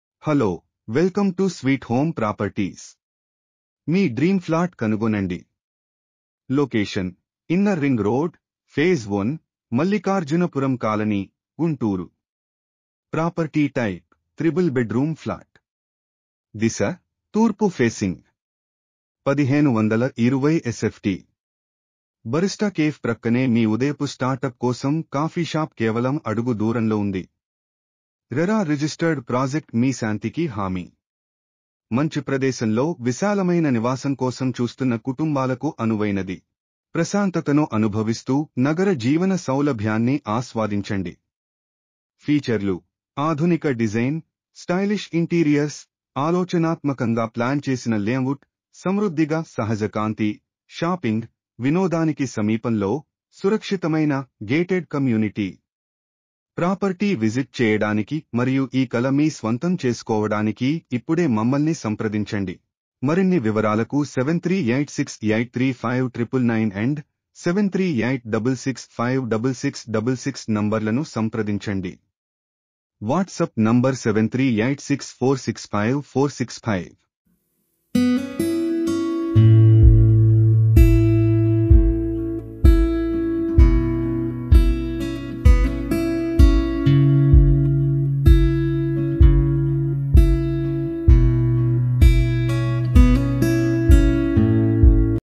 3Bhk ఫ్లాట్స్ మంచి ఇంటీరియర్ వర్క్ తో గుంటూరు మహాత్మాగాంధీఇన్నర్రింగ్ రోడ్డులో అమ్మబడును.
East Facing
 flats for Sale at 🛣️ Innerringroad,
phase 1
GUNTUR 🏢

👉3 bhk 1520sft

👉AP RERA approved Project
👉quality construction
👉Teak wood doors🚪and window frames🪟
👉 False Ceiling
👉living room
🚰Sufficient ground water🚿
👉 Backup supply for lift,water,common areas
👉 CCTV, fire & Safety
👉Lift & Generator🚗Car Parking